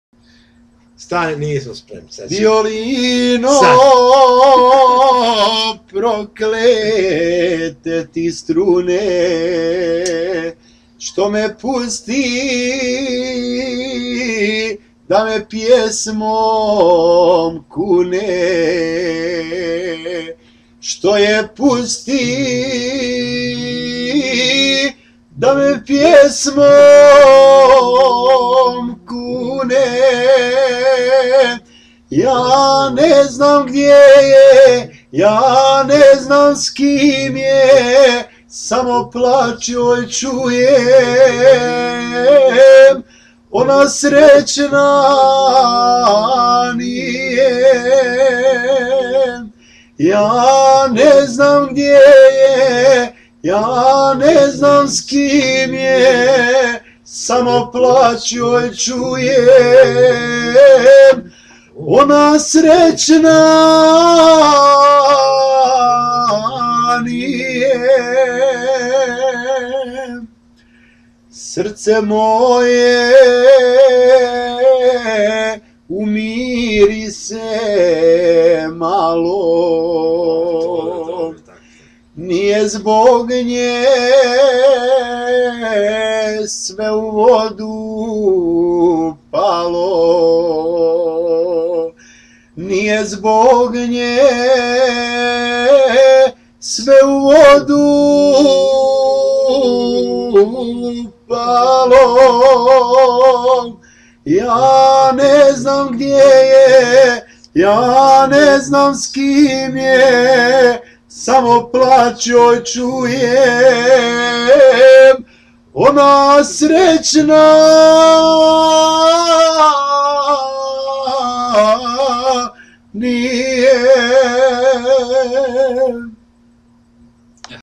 Enis Dedic (TurboDiesel) pjeva
Enis Dedic (TurboDiesel) pjeva sa Sanelom Ekic (TurboPlin) i Sead Delic (TurboFica)